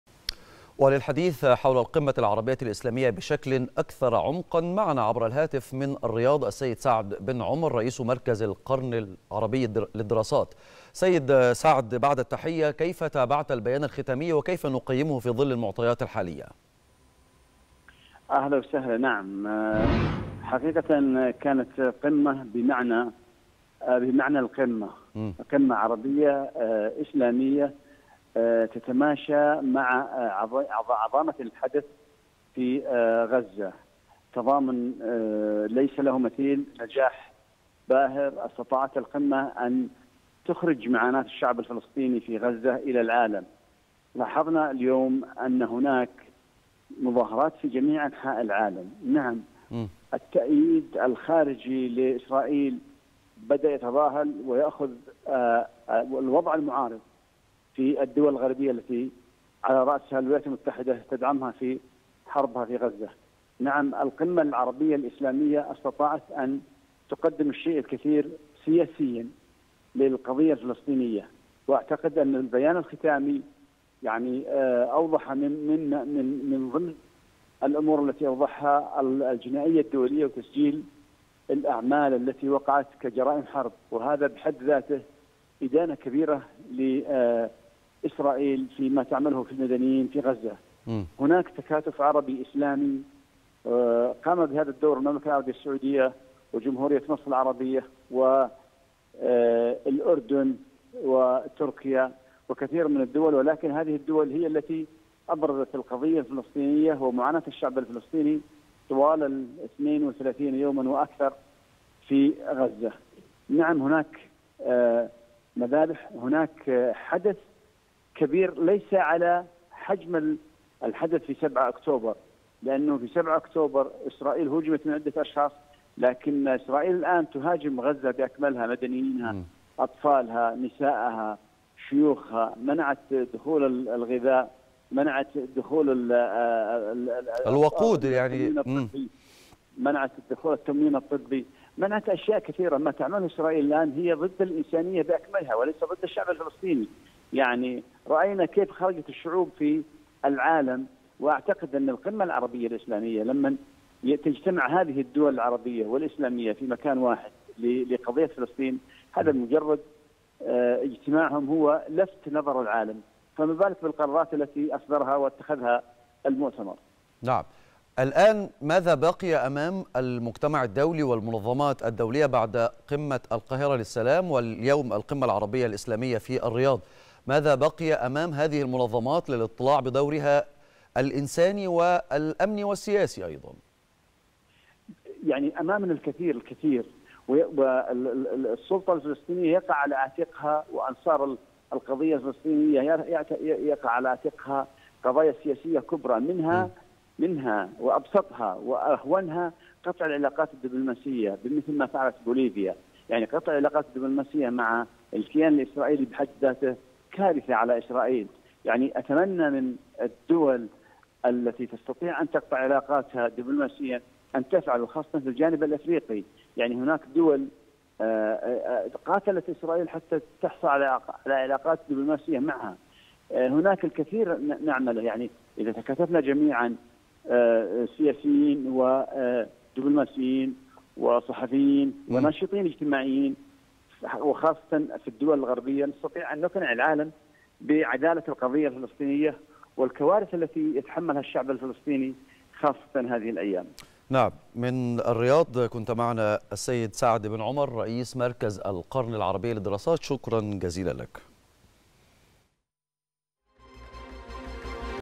سعد بن عمر: القمة العربية الإسلامية تنعقد في ظروف استثنائية يعيشها الفلسطينيون بقطاع غزة
سعد بن عمر رئيس مركز القرن العربي للدراسات: القمة العربية الإسلامية تنعقد في ظروف استثنائية يعيشها الفلسطينيون بقطاع غزة